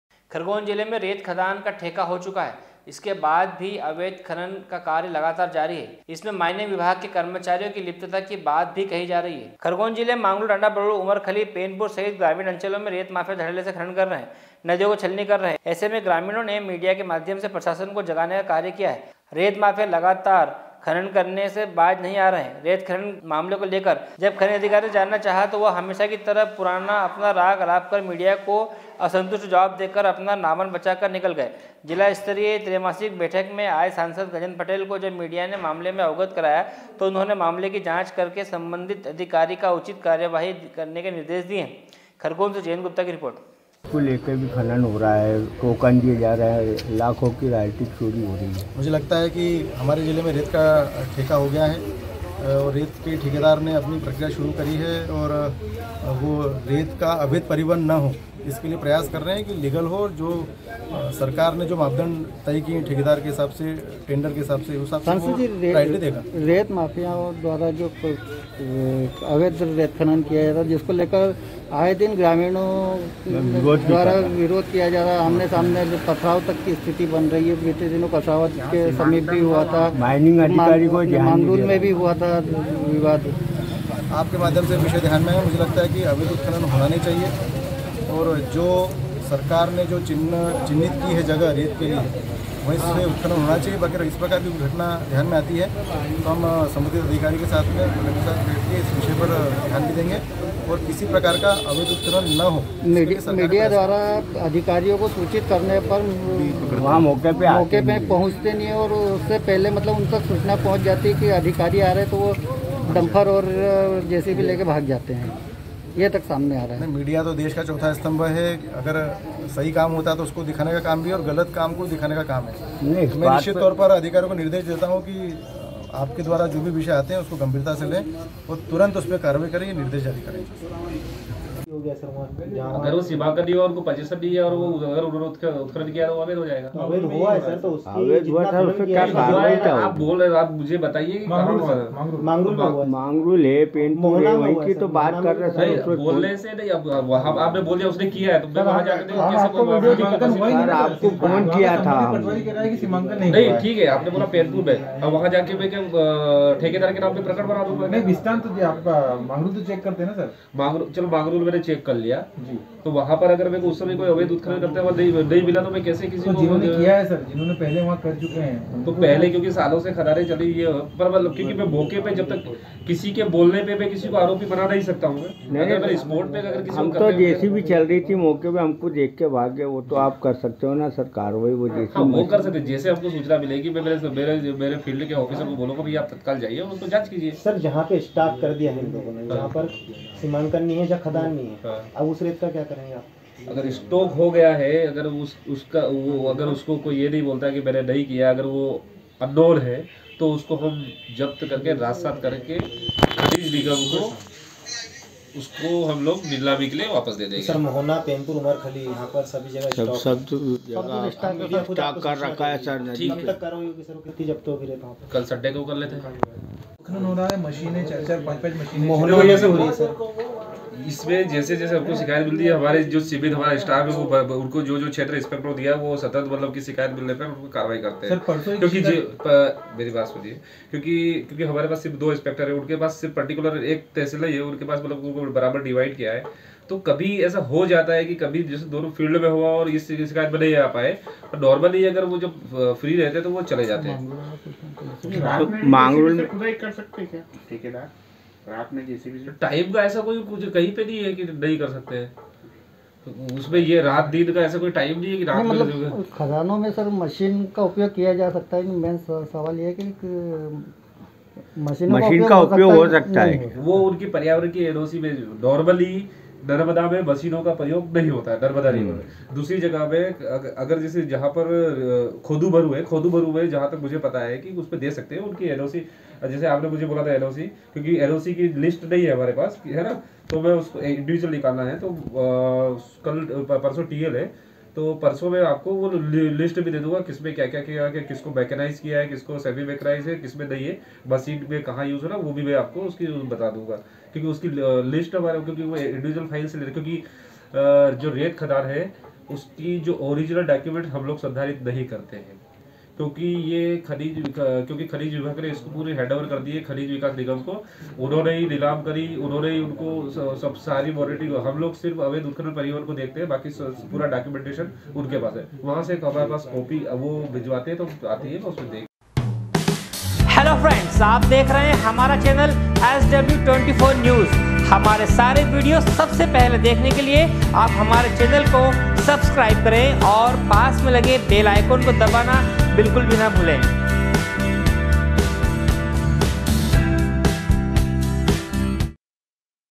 Khargone- खरगोन जिले में रेत खदानों का ठेका होने के बाद भी अवैध रेत खनन जारी है।
खरगोन जिले में रेत खदानों का ठेका होने के बाद भी अवैध रेत खनन जारी है। इसमें माइनिंग विभागकर्मचारियों की लिप्तता की बात भी सामने आई है। खरगोन से जयंत गुप्ता की रिपोर्ट